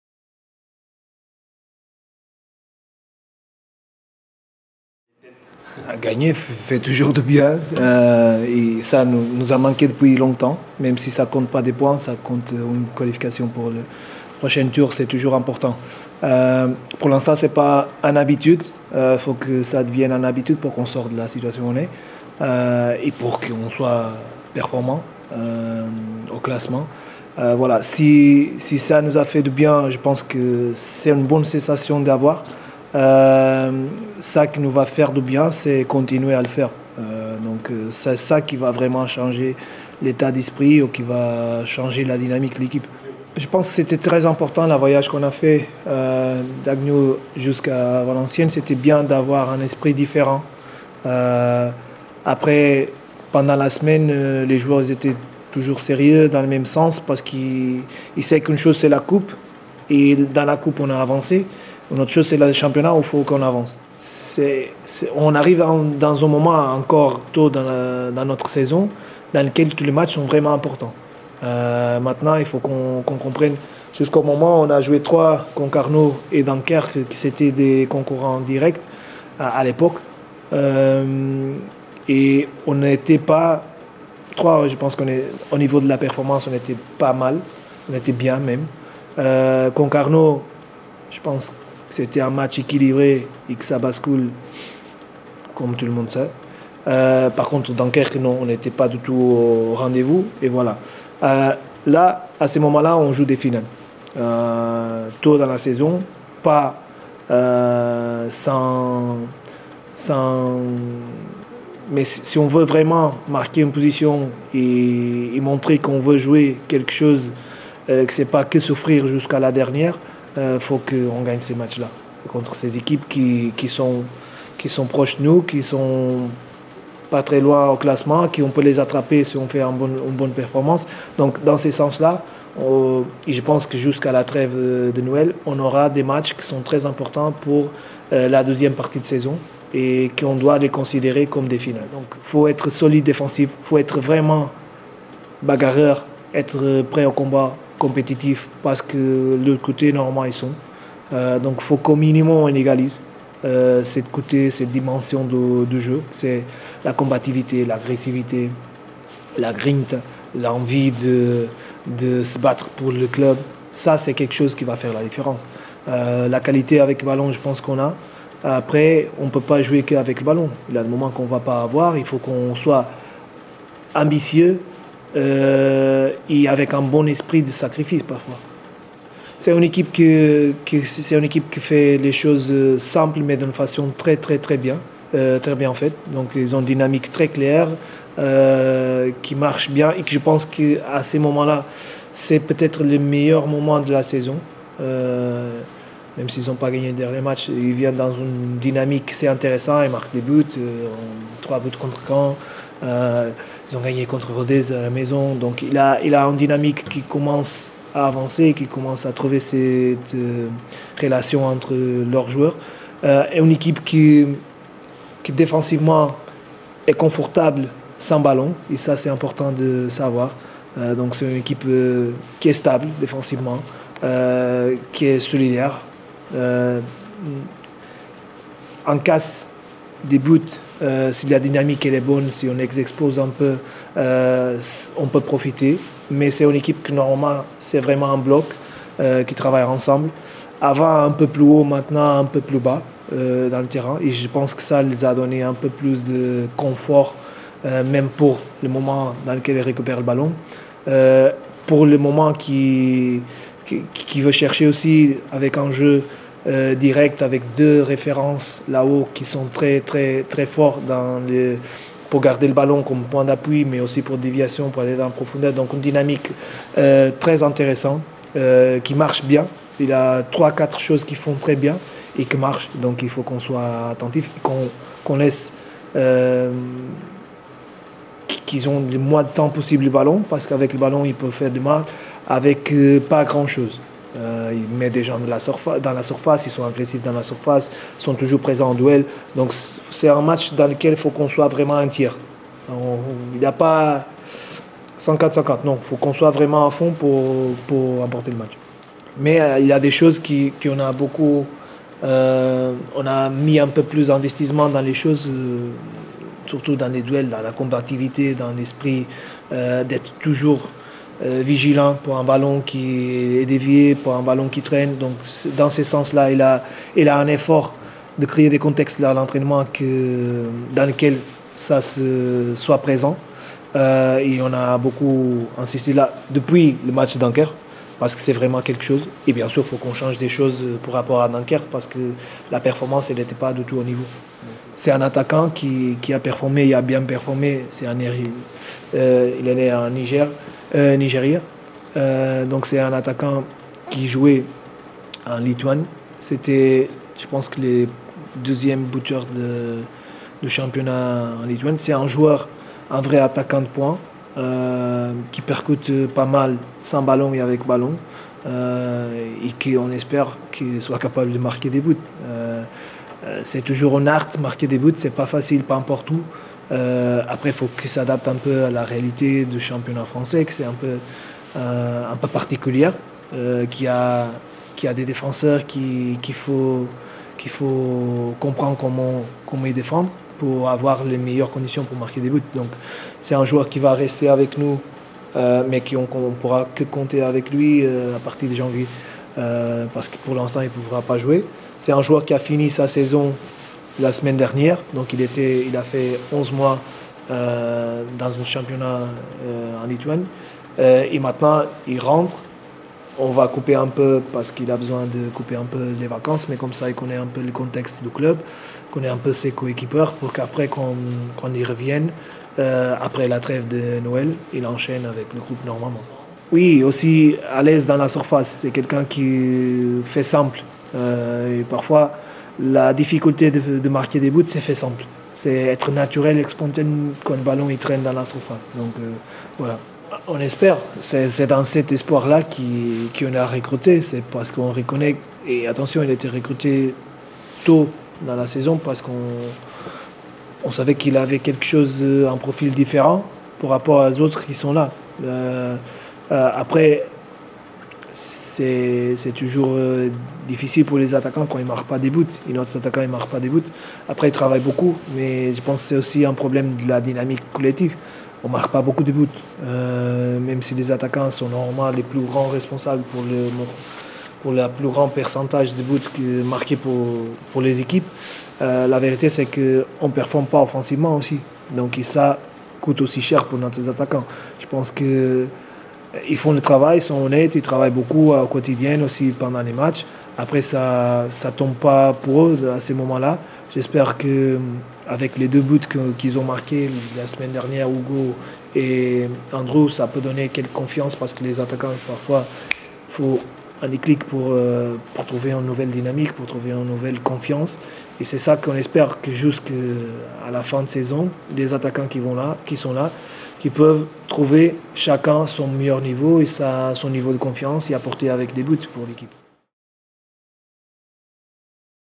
J15, QRM - VAFC | 🎙️ Conférence de presse d'avant match de Jorge Maciel
Retrouvez la conférence de presse du coach Jorge Maciel avant le déplacement à QRM.